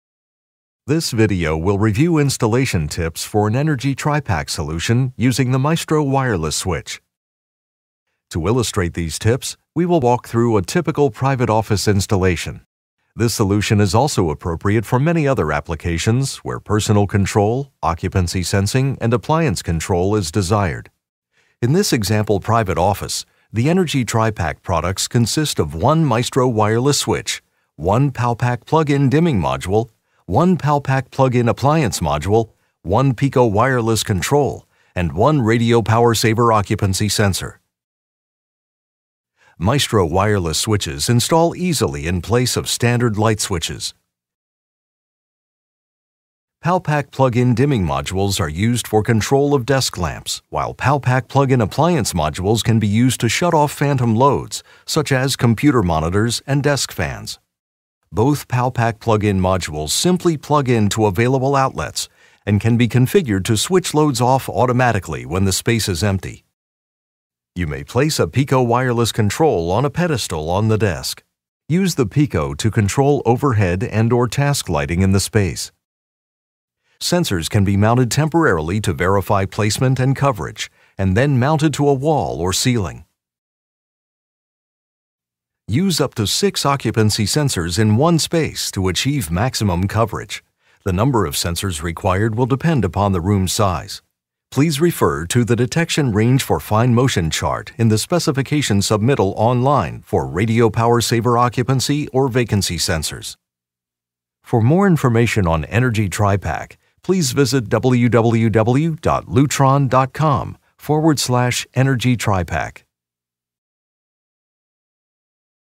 Energi TriPak: Private Office Installation Tips
See how simple is it to install an Energi TriPak system in a private office. Review components seen in a typical private office setting.